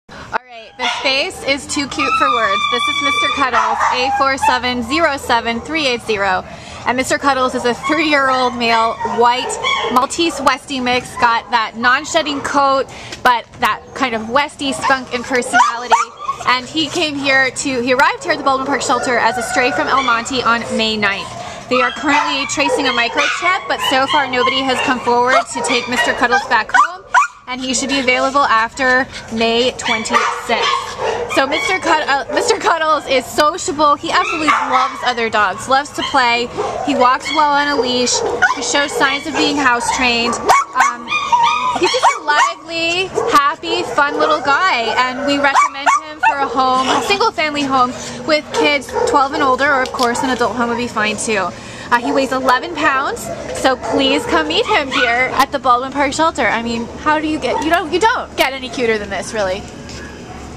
A4707380 Mr Cuddles
Mr. Cuddles is a peppy 3-year-old white male Westie/Maltese mix who was found in El Monte on May 9th and brought to the Baldwin Park Animal Care Center. Weighing 11 lbs, Mr. Cuddles has the looks of a dog from the cover of a Hallmark card ... and the personality to match. Sociable with people, he also loves the company of other dogs, walks well on leash, and shows signs of being housebroken. With a beautiful coat and floppy ears, Mr. Cuddles's curious and lively energy will fit best in an active household that will include him on walks, runs, hikes, and all sorts of other outdoor doggy adventures.